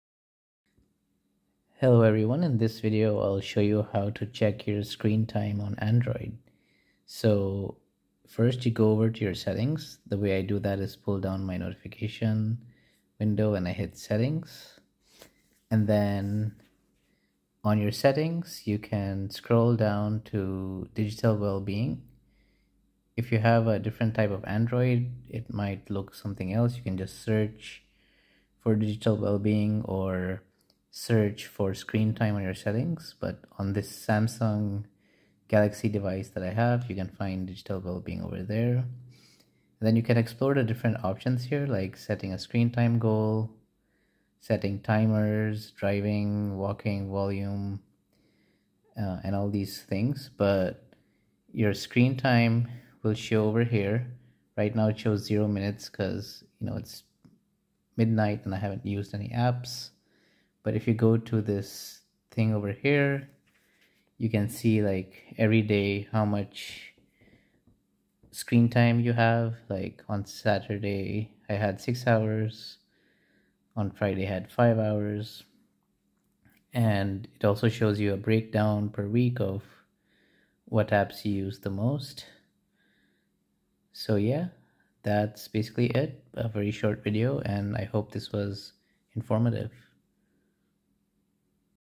How long are you staring at your Android😏
This video shows you how to check your screen time on an android device. I have a Samsung Galaxy S22.